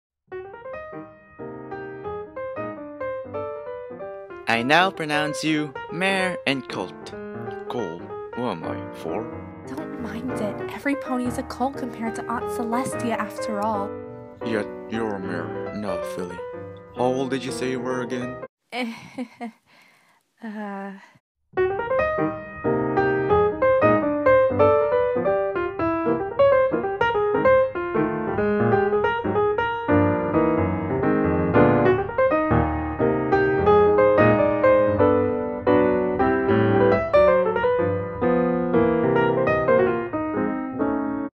"I Now Pronounce You" MLP comic dub
How old is Cadence?
My Little Pony: Friendship is Magic is owned by Hasbro
Cast:

Princess Celestia and Shining Armor- Me

Princess Cadence- Eliza Vandewalle